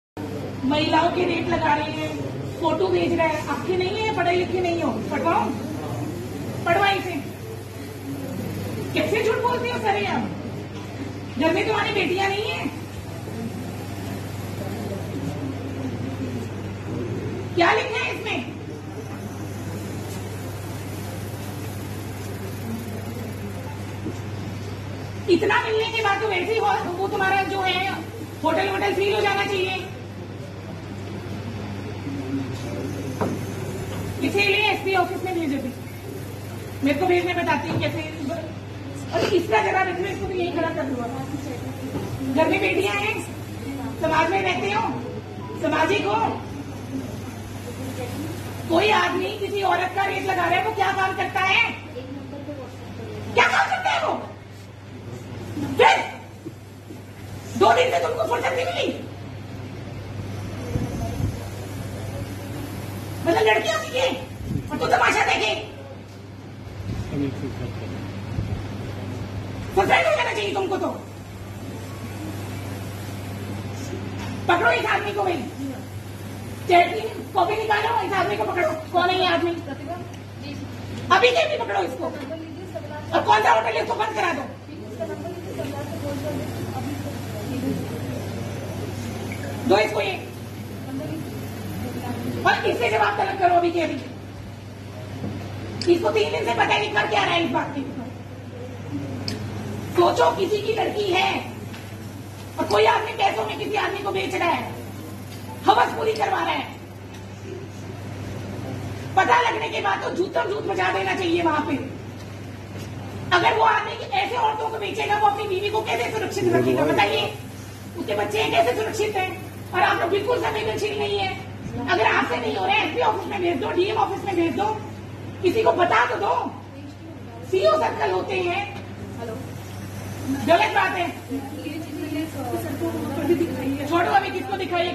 Meerut news ||State Commission for women||राज्य महिला आयोग उपाध्यक्ष Shusma singh की दरोगा को फटकार
मेरठ- राज्य महिला आयोग उपाध्यक्ष की फटकार.. सुषमा सिंह ने दरोगा को लगाई फटकार.. महिला की शिकायत पर सुषमा सिंह उपाध्यक्ष ने पुलिसकर्मी को जमकर डांटा।‼️

अधिक जानकारी.